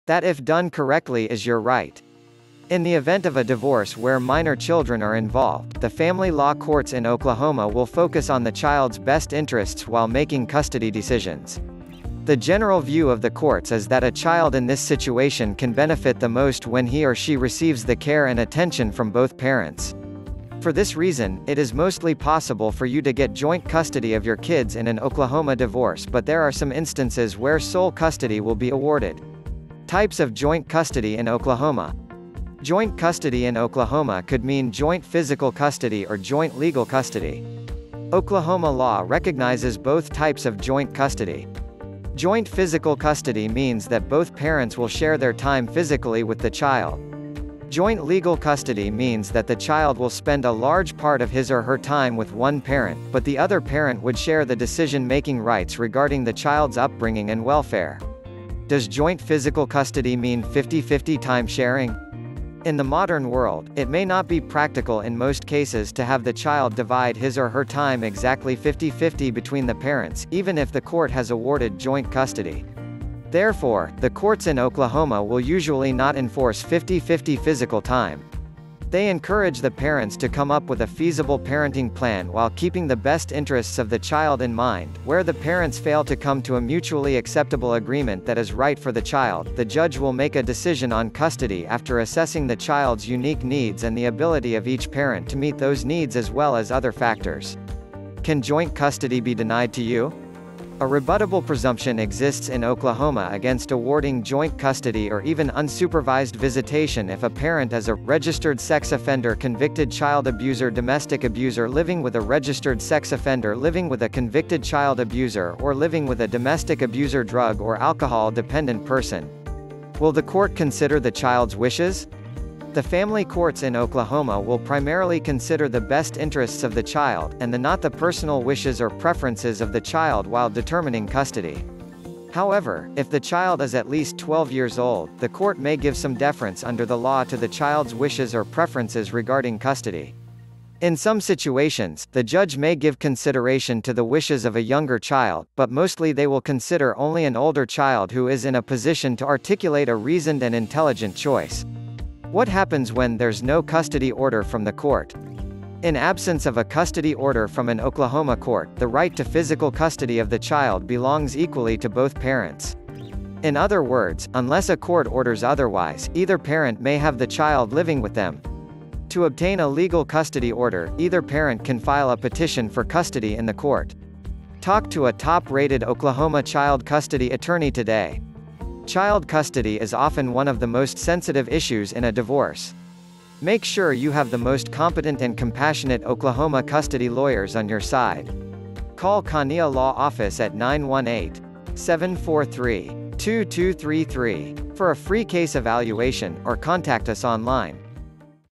Can I get joint child custody of my kids in Oklahoma? - Kania Law Office -
The type of custody you get of your children depends on several family law factors. You can get joint child custody and Oklahoma family Courts do this all the time.  Charles Kania is a joint custody attorney in Tulsa. He practice custody law at Kania Law Office in Oklahoma.